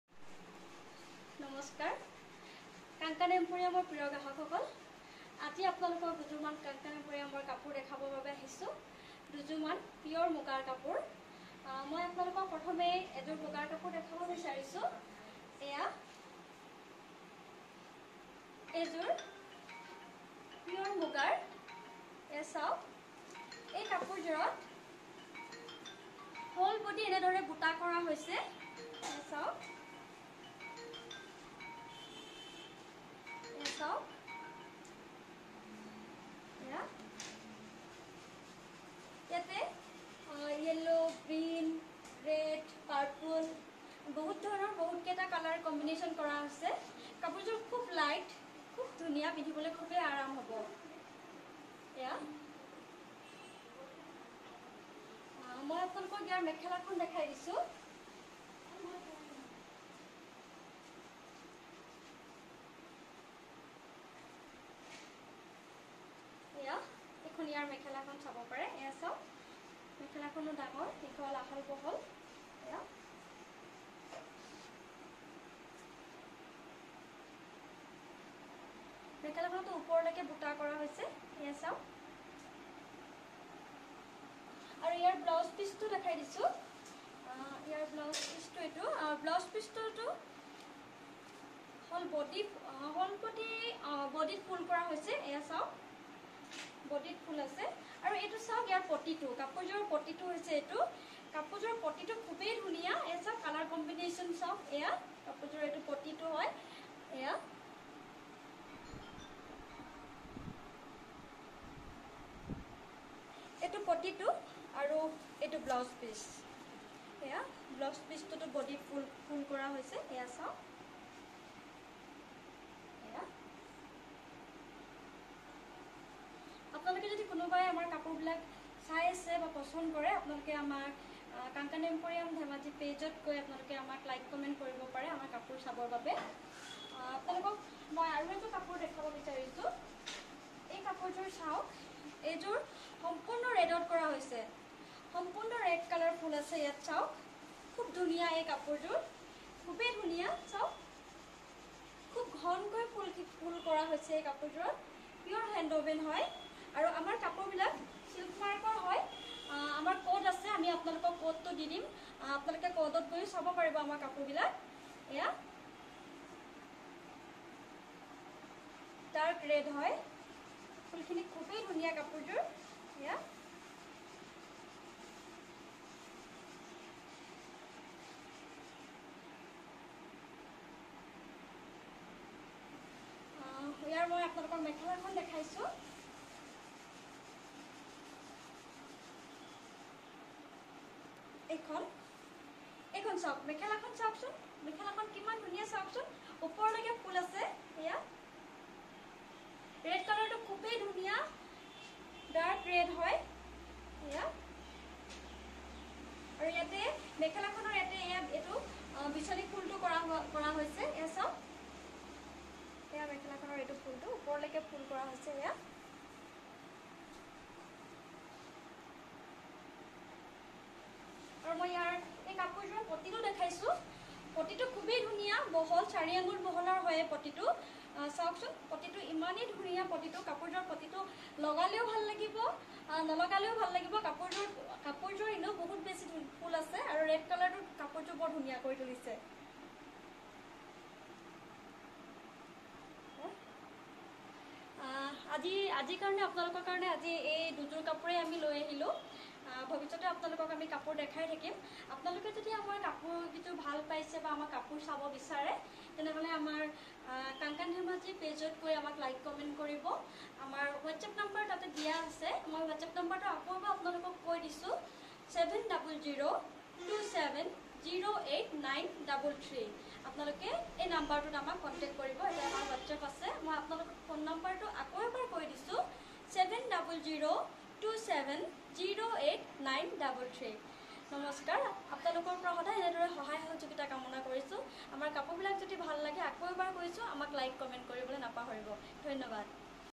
Pure Muga silk Mark
Hand woven pure Muga silk Mark Sadar makhlea with blouse piece and side poti